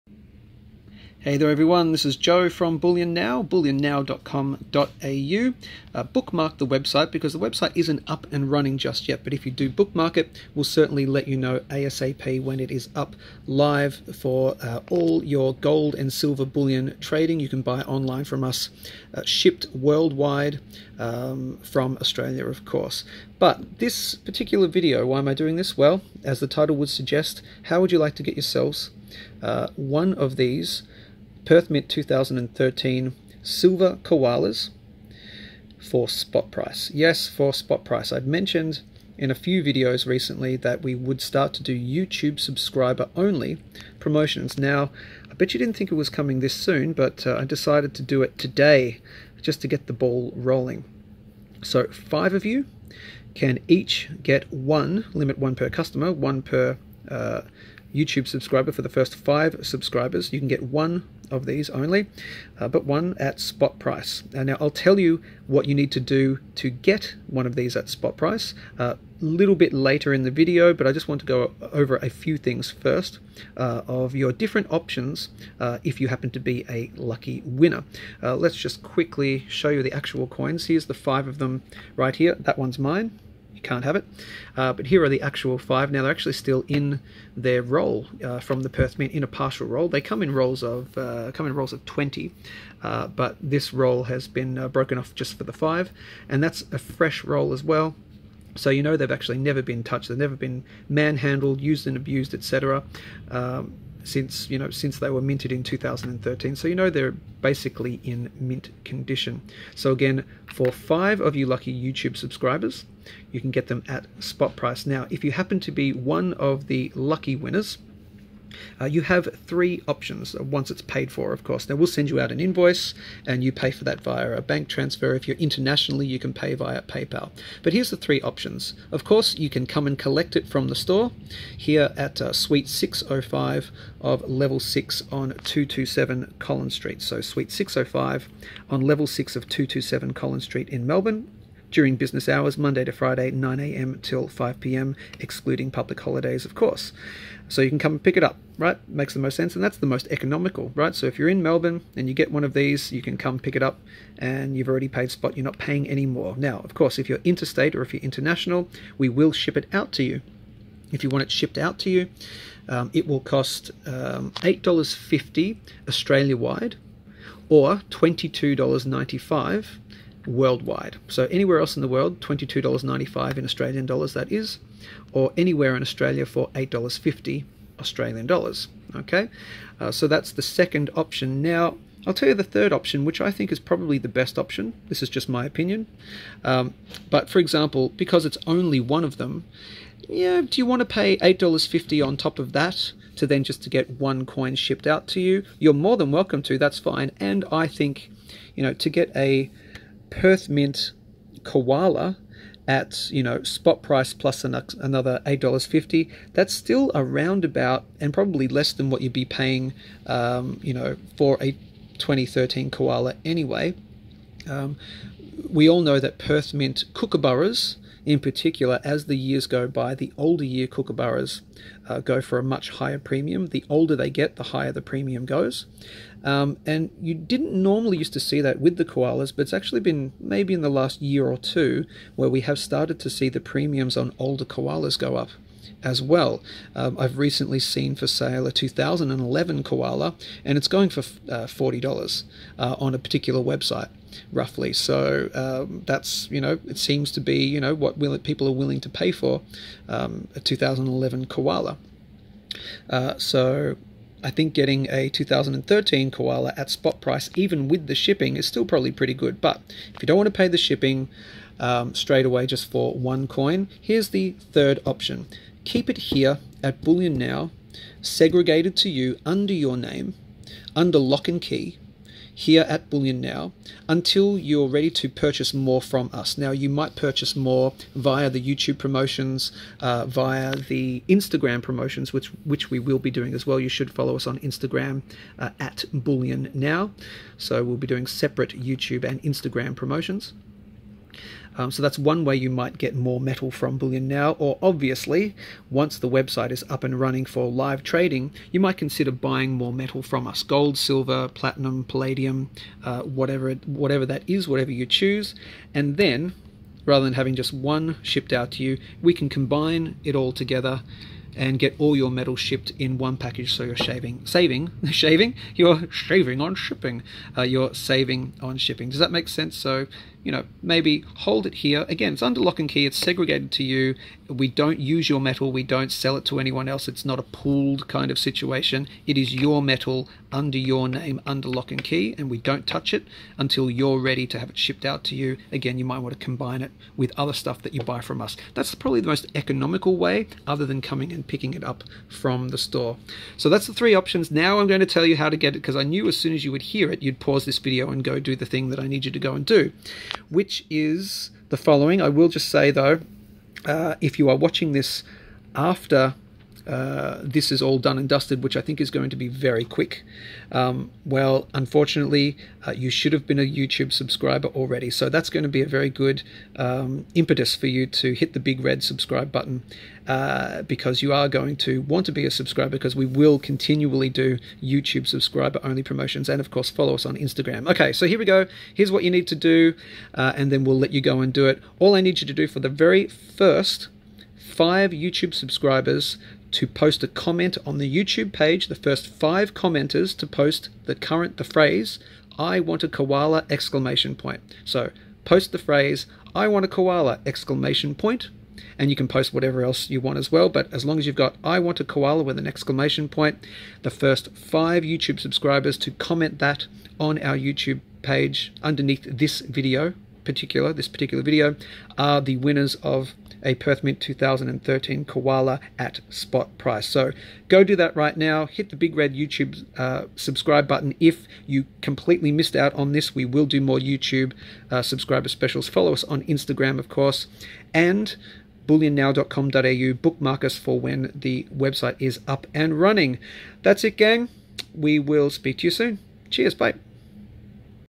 Want a Perth Mint Koala at SPOT PRICE? | Bullion Now YouTube Special
Get a 2013 Perth Mint Koala Silver Coin at SPOT PRICE in our first YouTube SUBSCRIBER ONLY special. Gotta watch the video to find out what it is!

227 Collins St
Melbourne